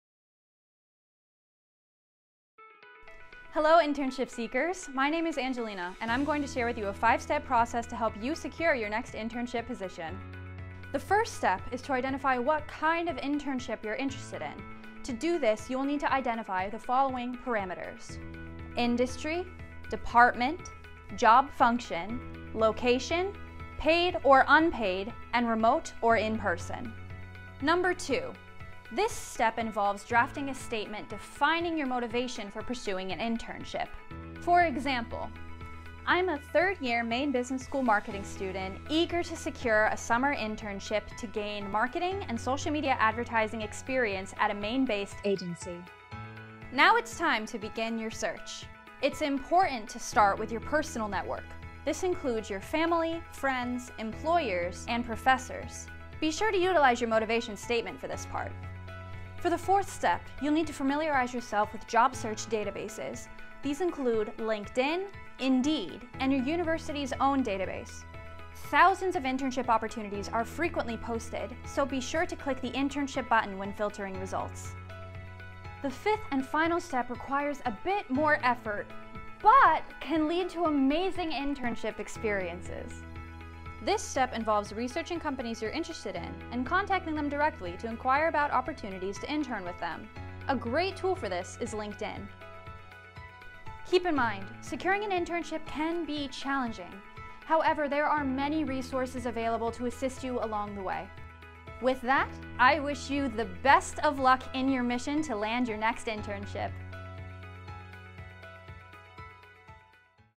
Searching for Internships
This video highlights three strategies for searching for internship opportunities.

Credits: Angelina Buzzelli ‘20 & ‘22G, Nicholas Poulin ‘23, Melanie Brooks, Tanya Beaulieu, Taylor Ashley